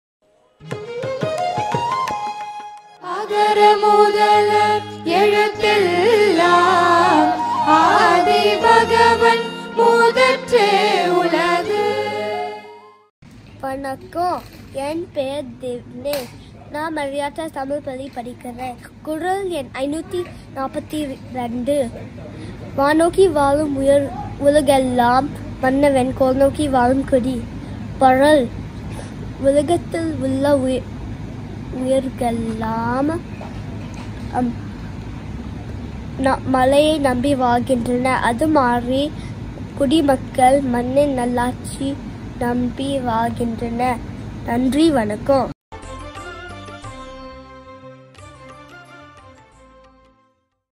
114 திவ்னேஷ் முருகானந்தம் Thirukural Neram - குறள் # : 542
அதிகாரம்: செங்கோன்மை
குறள் # : 542

குறள் : வானோக்கி வாழும் உலகெல்லாம் மன்னவன்
கோல்நோக்கி வாழுங் குடி